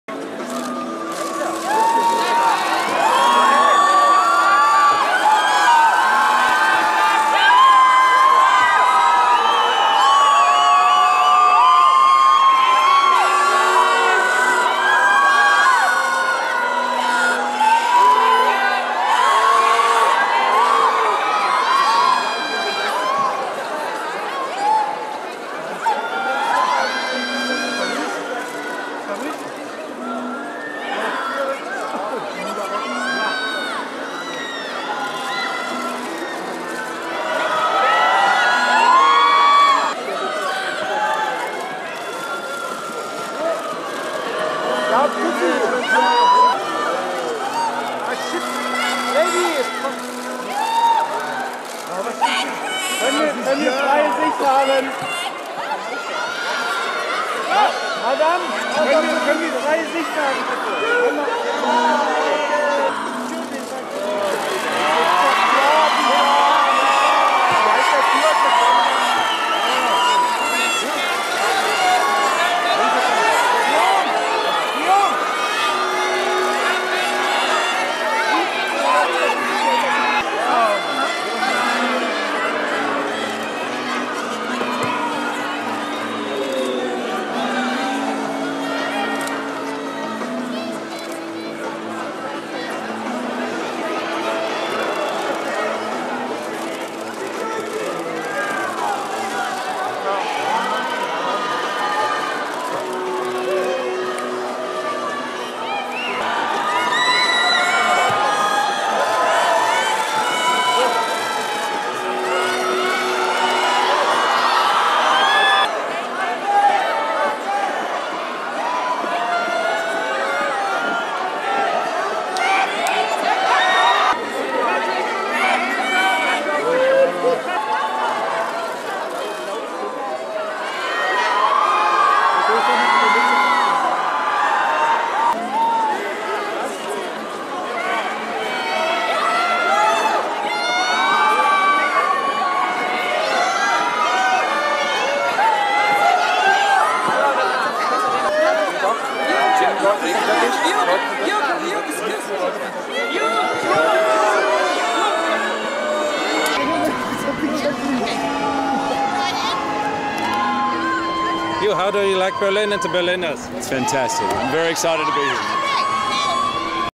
Hugh Jackman surprises his fans @LOGAN WORLD PREMIERE
Hugh Jackman signing autographs for his fans at Berlinale Film Festival. It's the world premiere of "LOGAN - The Wolverine".